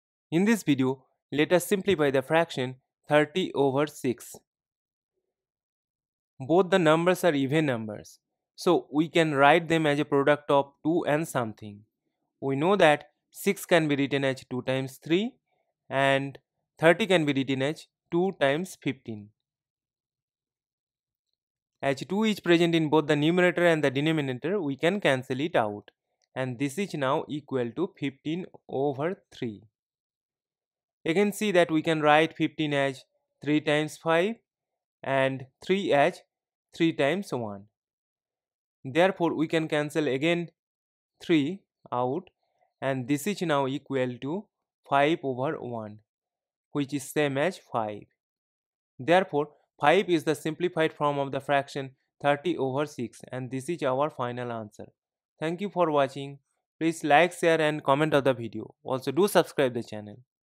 How to simplify the fraction 30/6 | 30/6 Simplified
Topic: How to simplify the fraction 30/6. 30/6 in its simplest form. 30/6 simplified.

In this video, we write down the fraction 30/6 in its simplified form.

Q: What is 30/6 simplified.

A: Note that 5 is the simplified reduced form of 30/6.

We post videos daily on youtube